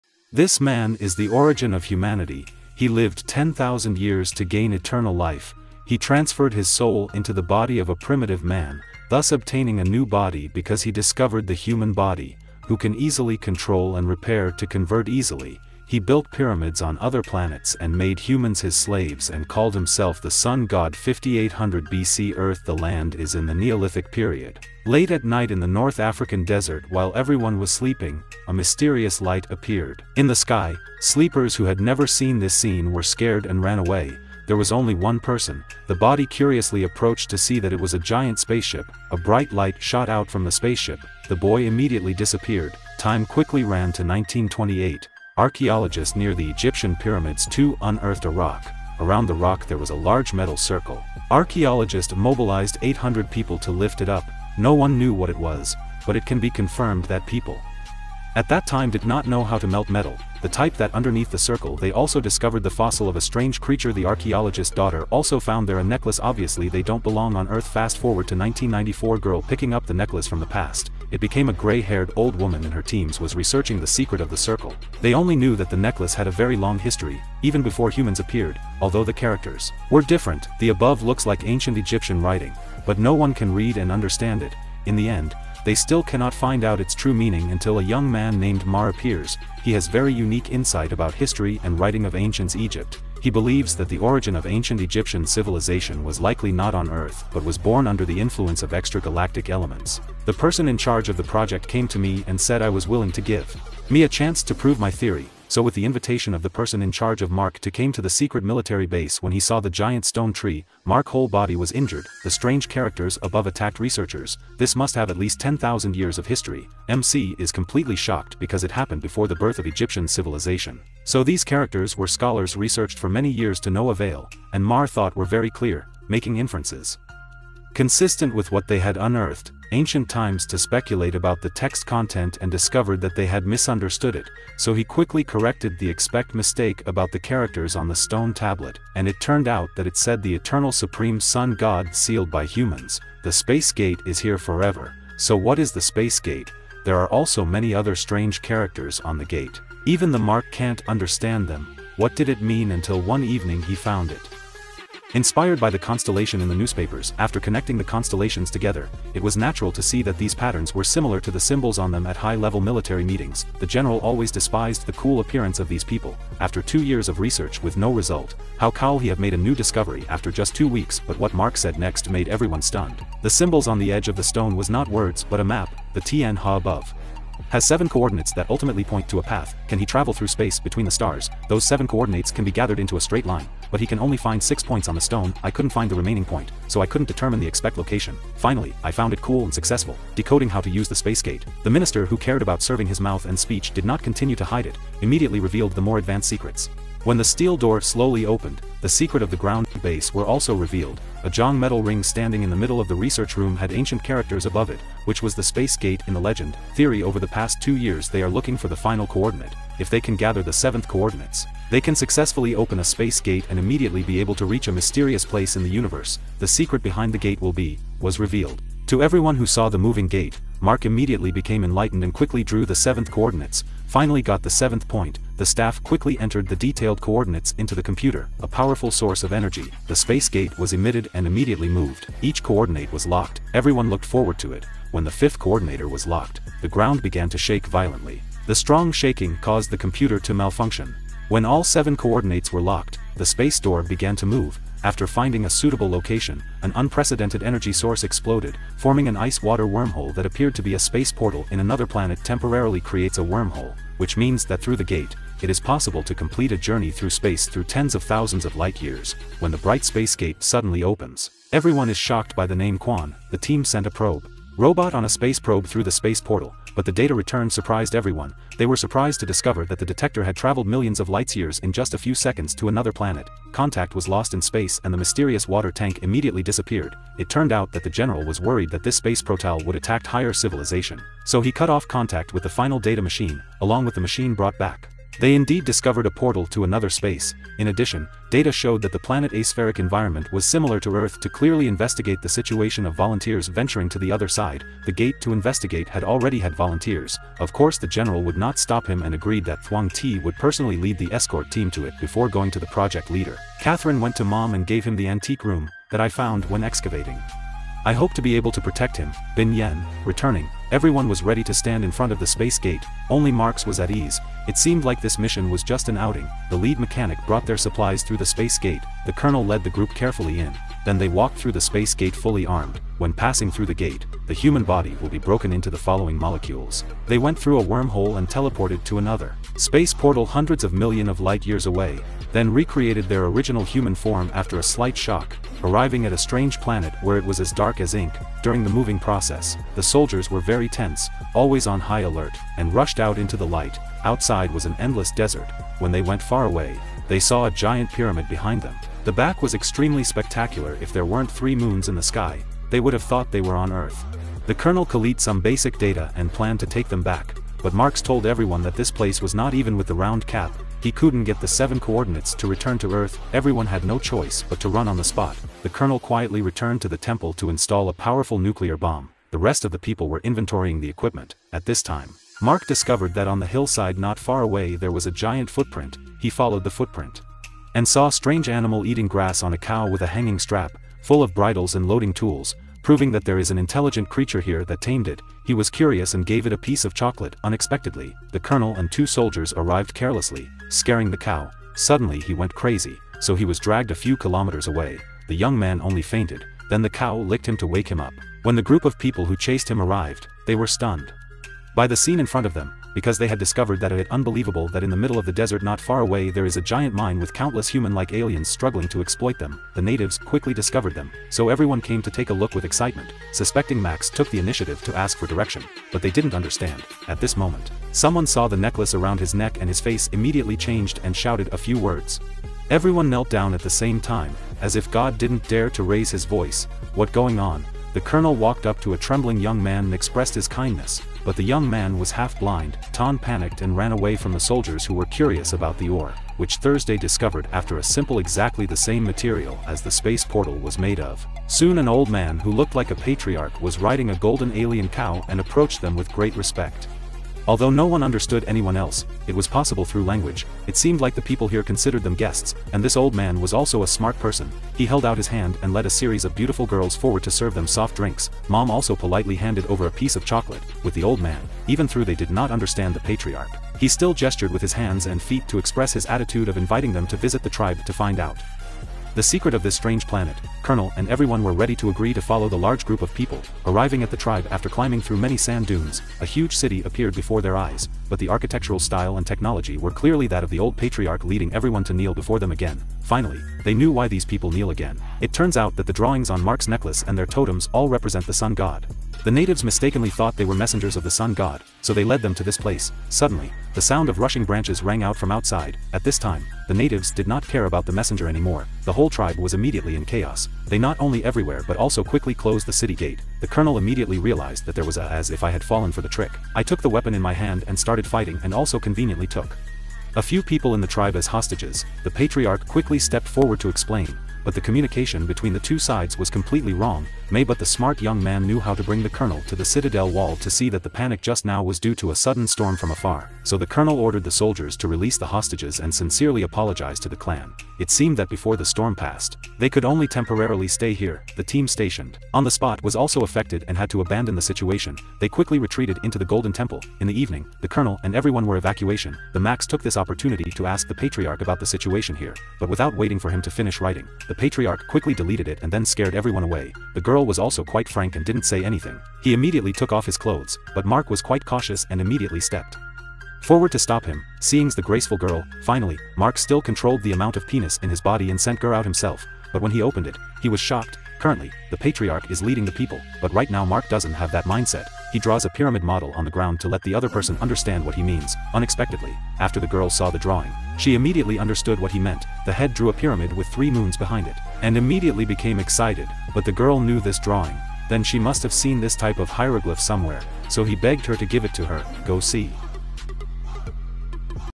[Movie Review] The Pyramid Is a Spaceship Leading to the Planet of the Gods...
Movie content: Heaven's Gate - An interstellar teleportation device, found in Egypt, leads to a planet inhabited by people like the ancient Egyptians worshiping the god Ra